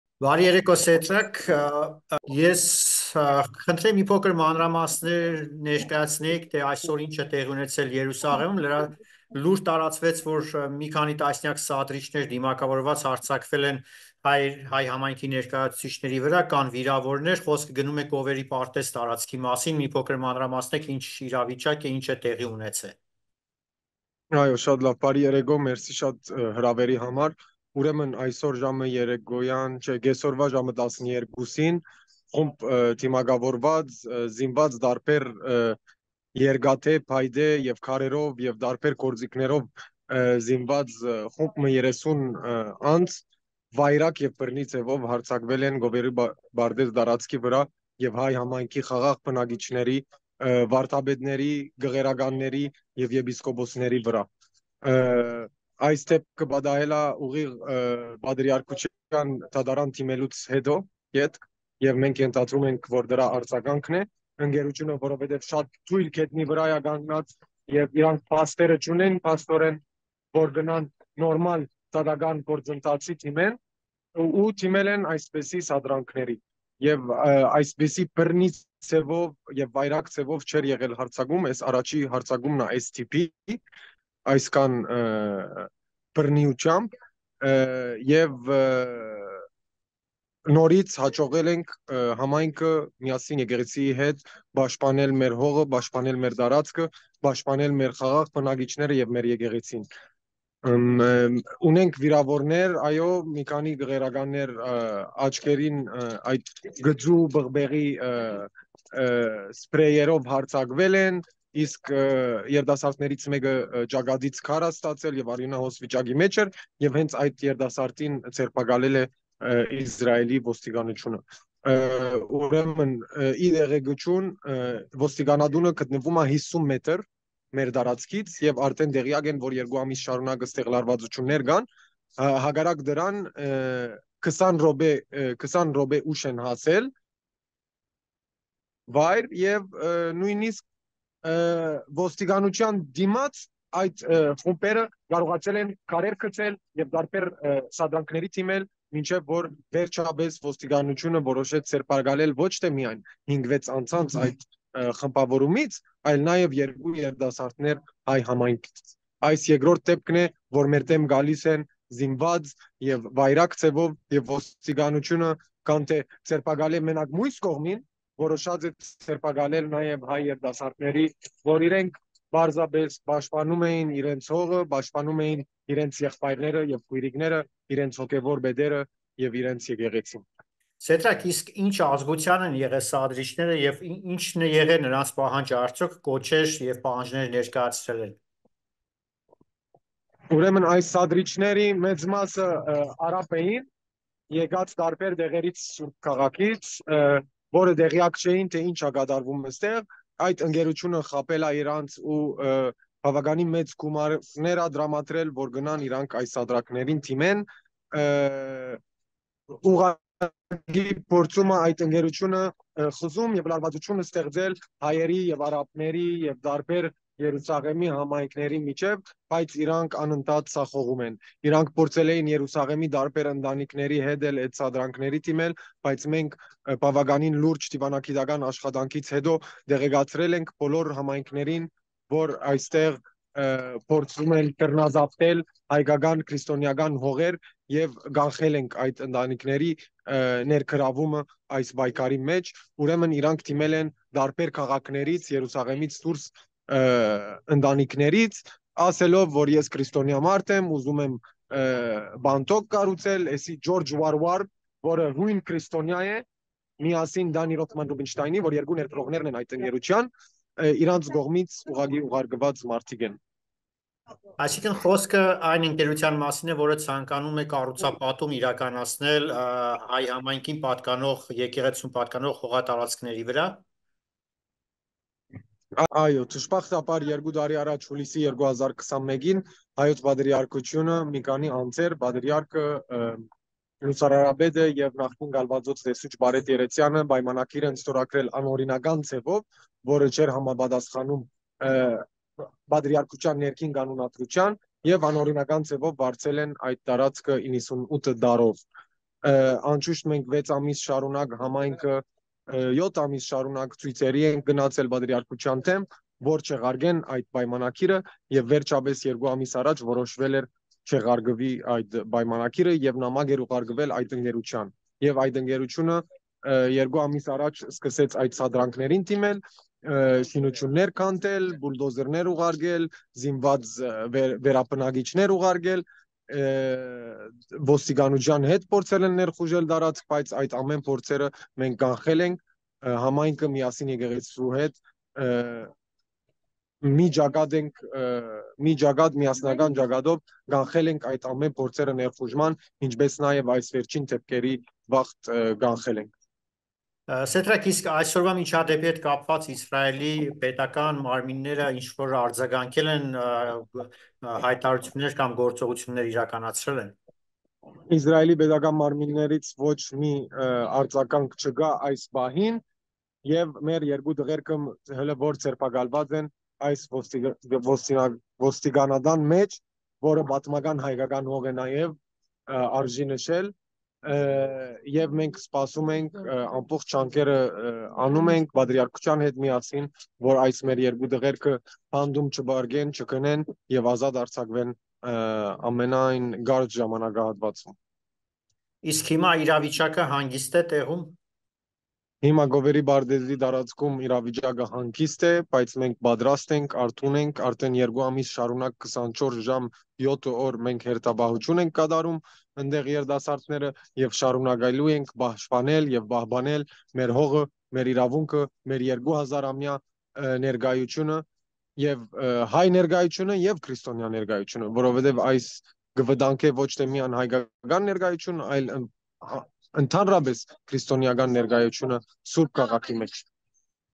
Երուսաղեմում սադրիչները հարձակվել են հայերի վրա(Սեդրակ Բալյան Երուսաղեմի հայ համայնքի ներկայացուցիչ)
🔷  ՀԵՏԵՎԵՔ ՄԵԶ ՆԱԵՎ ՊԱՇՈՏԱՆԱԿԱՆ ԿԱՅՔՈՒՄ 🔷